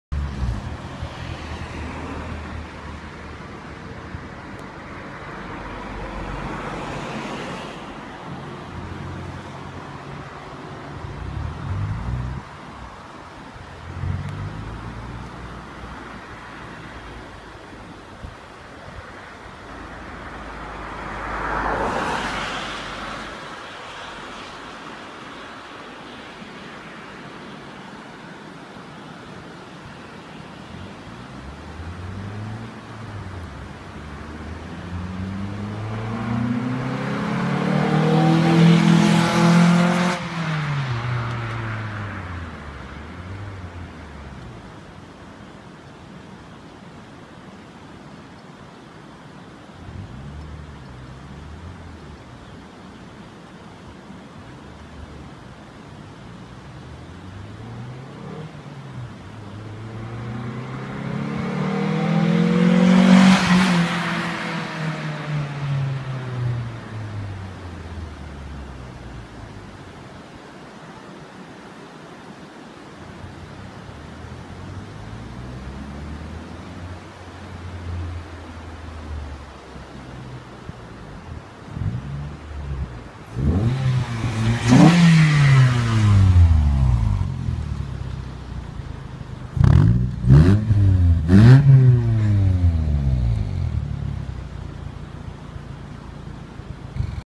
Toyota Celica 1.8 vvti japspeed exhaust and k&n short ram air intake
K&N short ram air intake and japspeed exhaust with db killer, without was toooooo loud! 😉😃